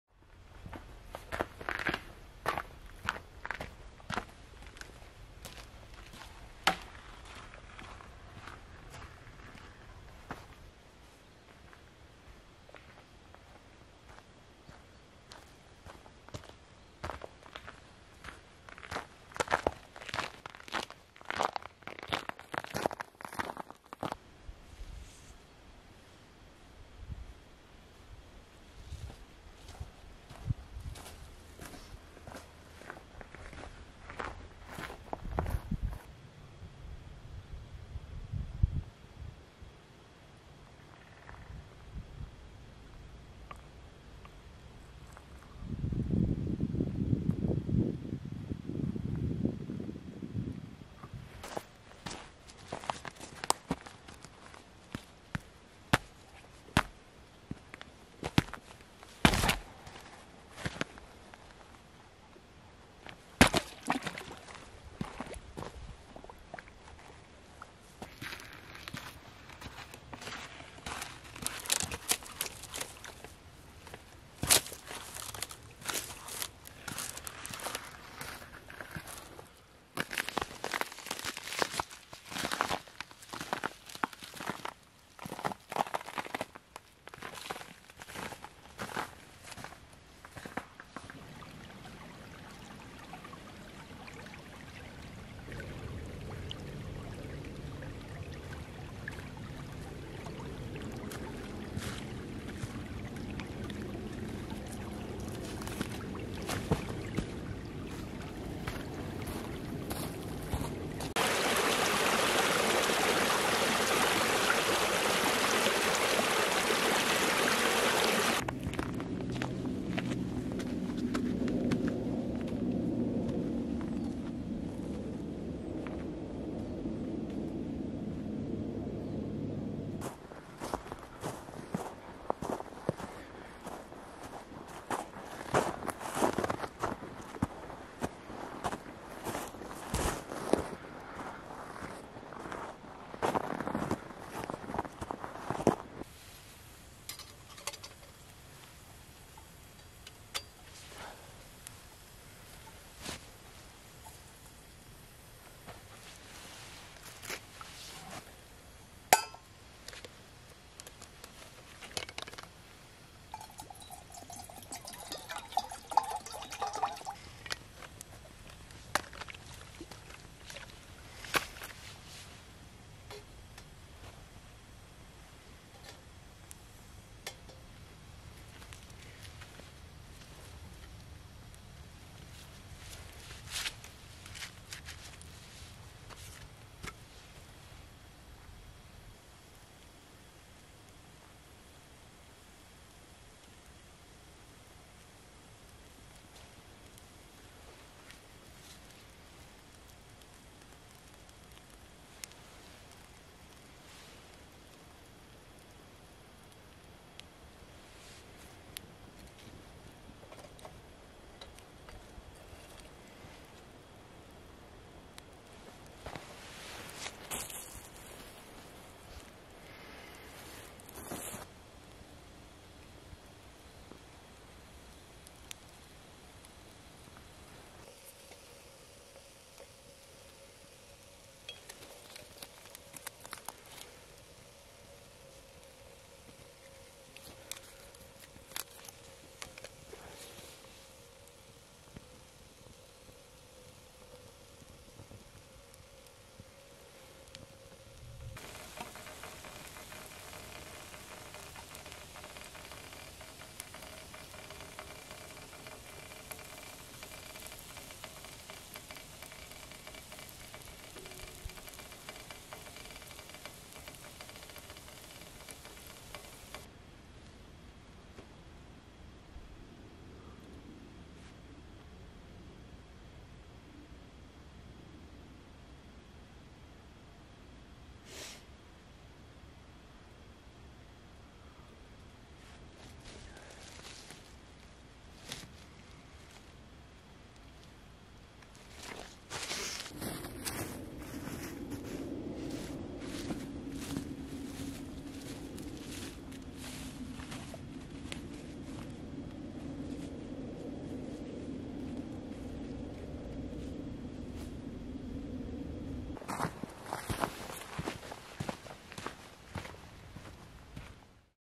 Hiking on Cold and sunny winter day - Warming up with Hobo stove
Hiking in National park

Osprey Hikelite 26, Varusteleka Hobo stove, Stainless canteen